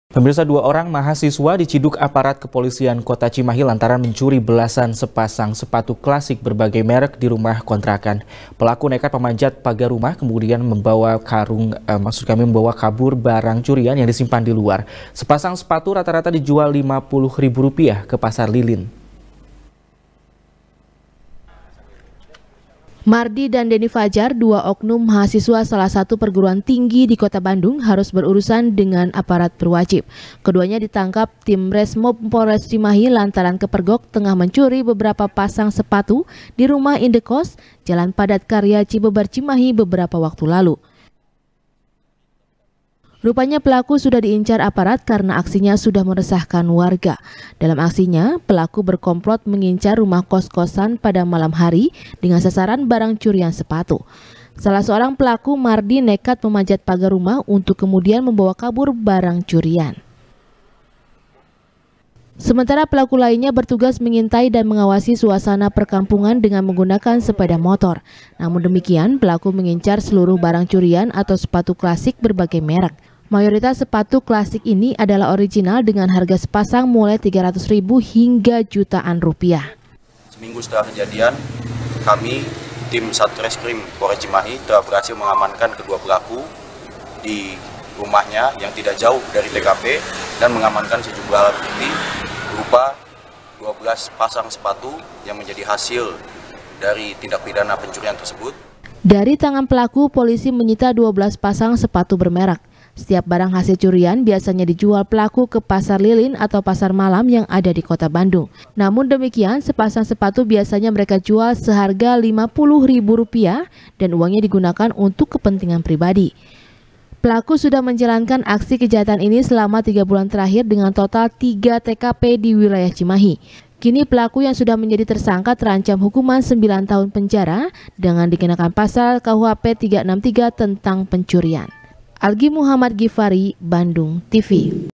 Dua Mahasiswa Pencuri Sepatu Dibekuk Satreskrim Polres Cimahi | SBR | BANDUNGTV
SBR | BANDUNGTV | IRFAN RAMANDA | Dua Orang Mahasiswa Diciduk Aparat Kepolisian Kota Cimahi, Lantaran Mencuri Belasan Pasang Sepatu Klasik Berbagai Merek Di Rumah Kontrakan. Pelaku Nekad Memanjat Pagar Rumah, Kemudian Membawa Kabur Barang Curian Yang Disimpan Diluar. Sepasang Sepatu Rata-Rata Dijual 50 Ribu Rupiah Ke Pasar Lilin.